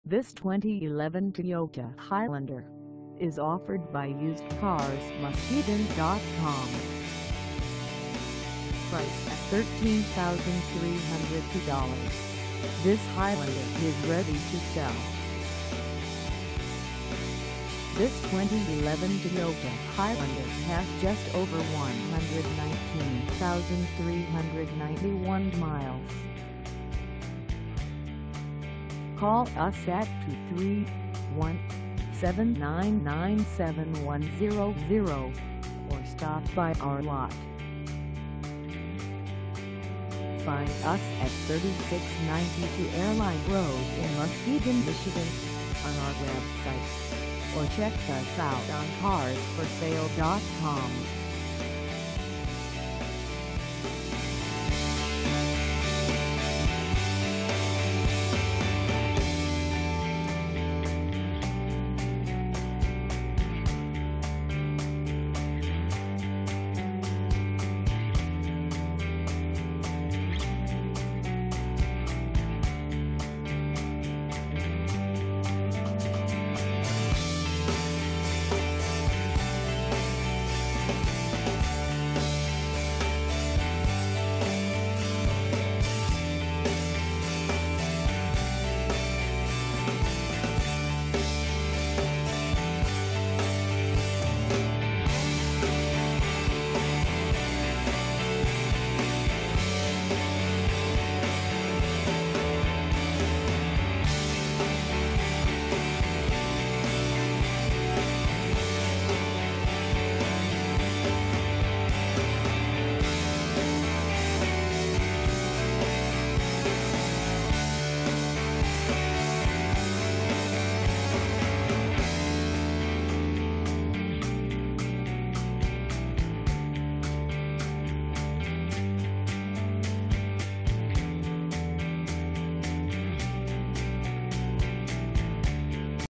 2011 Toyota Highlander for sale in Muskegon, MI 49444 at Us
This 2011 Toyota Highlander  is for sale in Muskegon, MI 49444 at UsedcarsMuskegon.com.

Engine: 3.5L V6
Transmission: Automatic 5-Speed
Color: Nautical Blue Metallic
Mileage: 119391
Contact UsedcarsMuskegon.com 3692 Airline Rd. Muskegon, MI 49444 to test drive this 2011 Toyota Highlander  today.